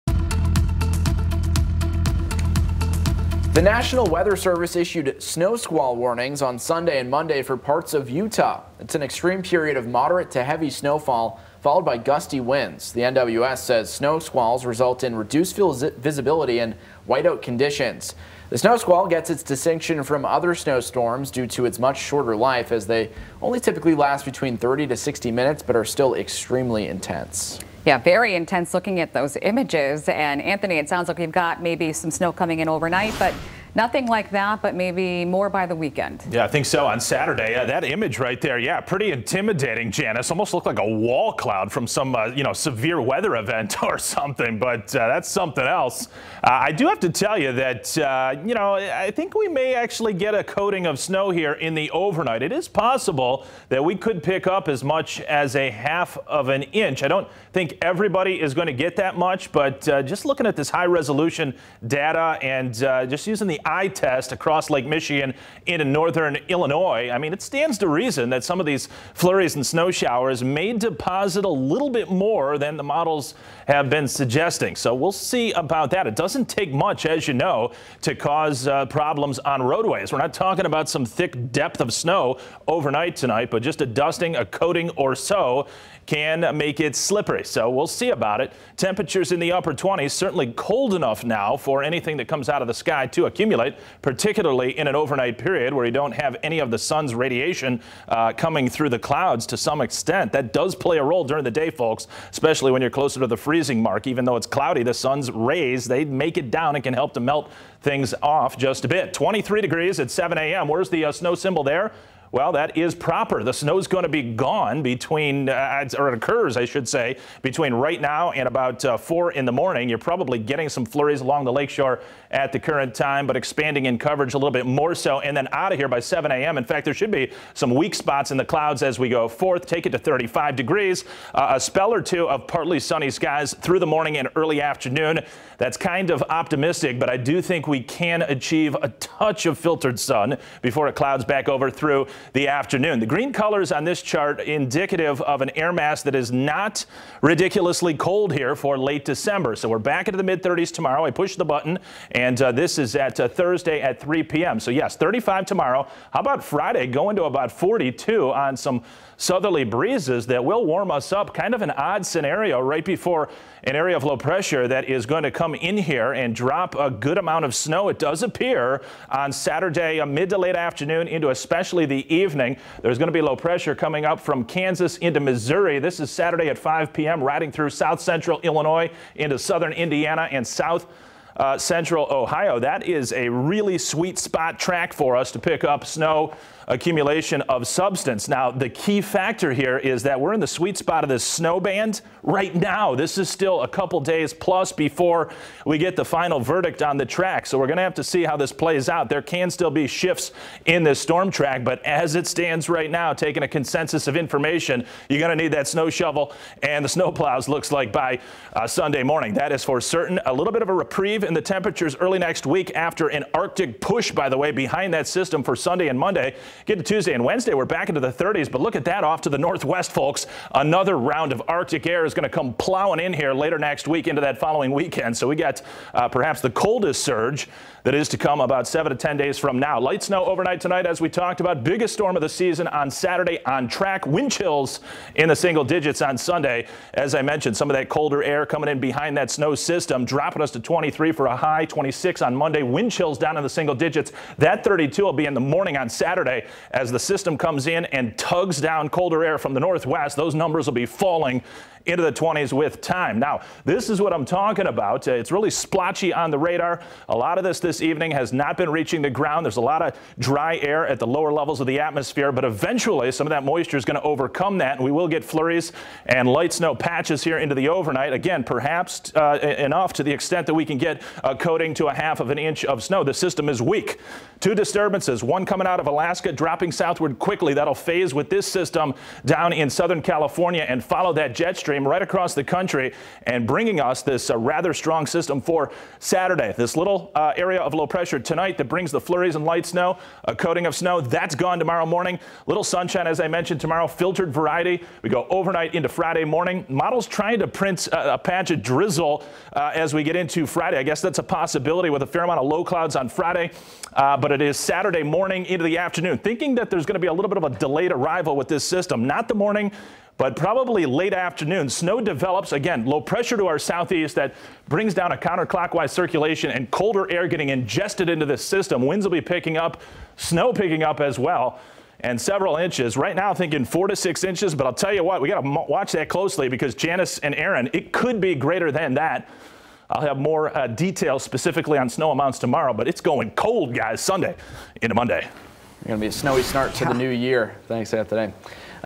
Wednesday Evening Forecast Dec 29, 2021
Significant snow potential Saturday P.M.